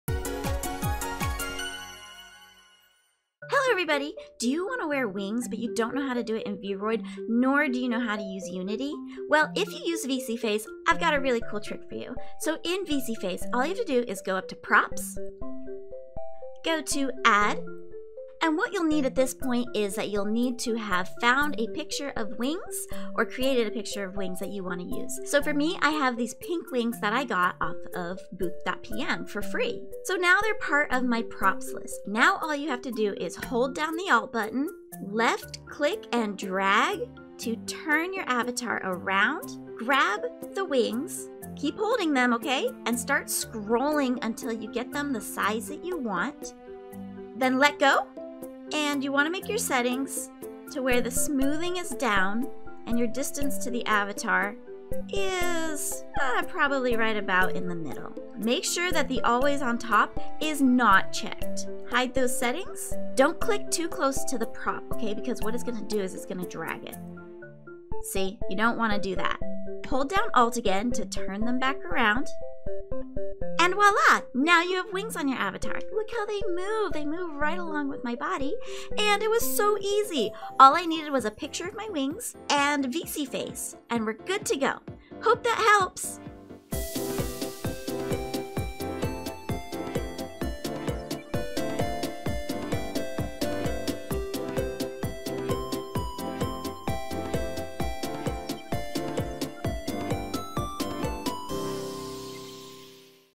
Put Wings on your Avatar using ONLY VSeeFace!
This quick tutorial will show you have to put wings on your avatar using only the facial capture software, VSeeFace. All you will need is a PNG picture of the wings you want to use and the program VSeeFace!

💚Thursday: 730pm-1030pm PT💚
💚Friday: 7pm-10pm PT💚
💚Sunday: 2pm-5pm PT💚

Lots of Love to all of you! I hope this video is helpful!

Find wings here: booth.pm